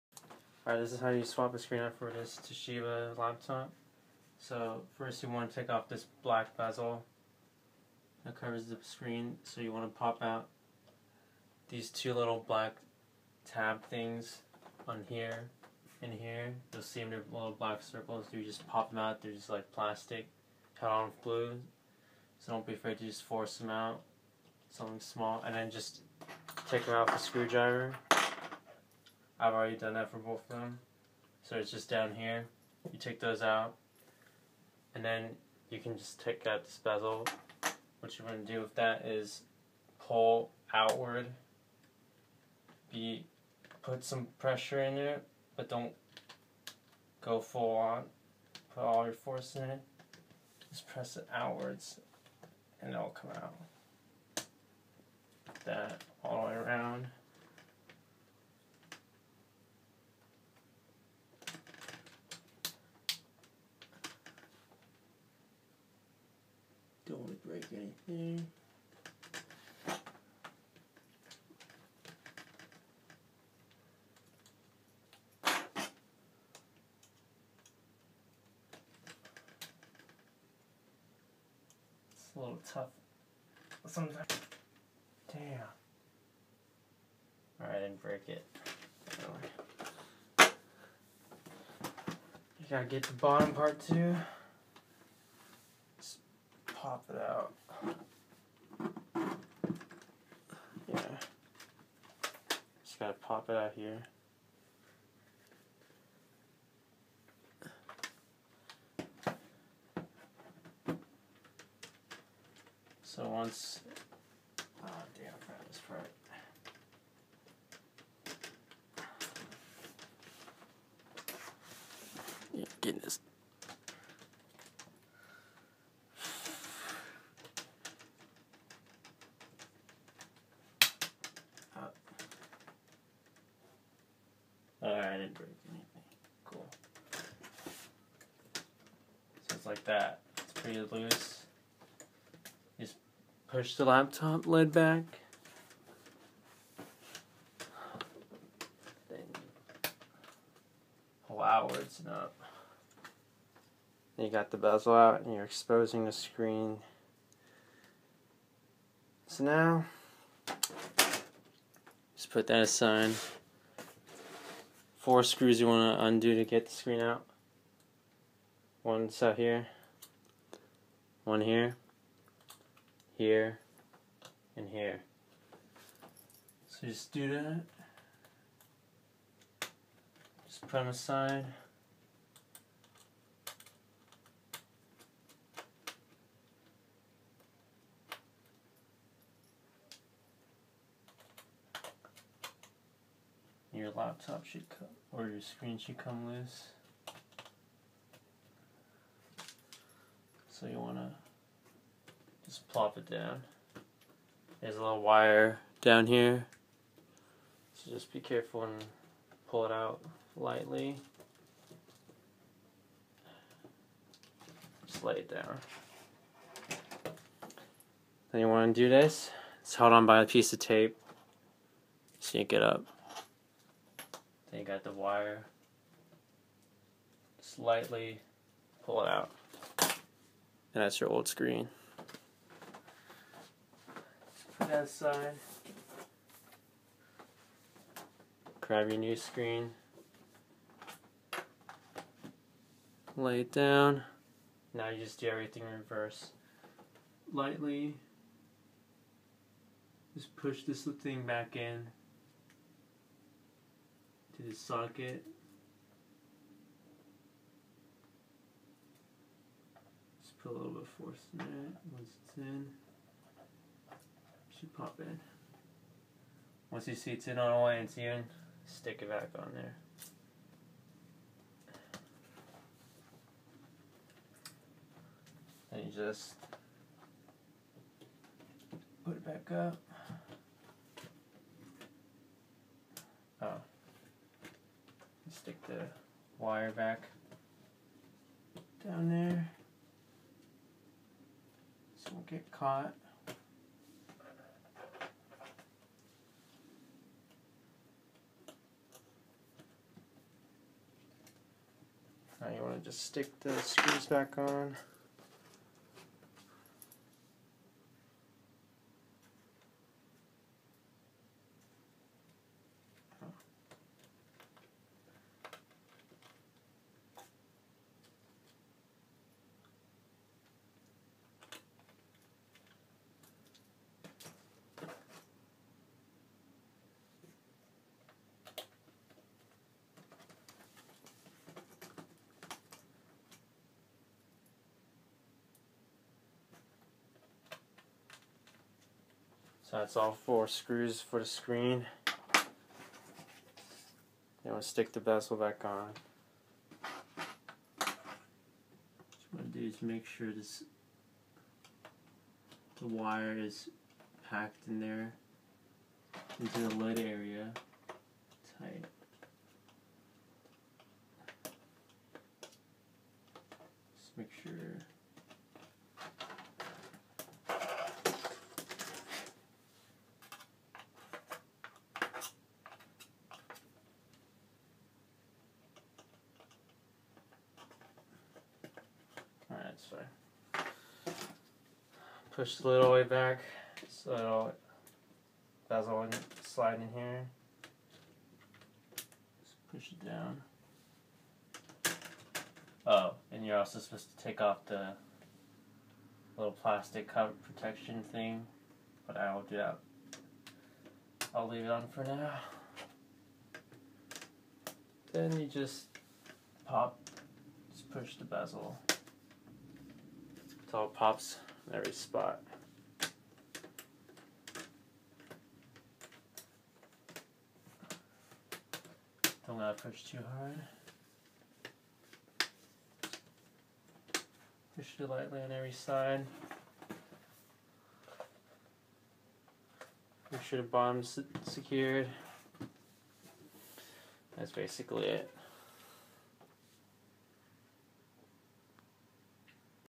Laptop screen replacement / How to replace laptop screen Toshiba Satellite P55-A5200
Replacing a damaged screen on a:
Toshiba Satellite P55-A5200

Turn on annotations for onscreen instructions.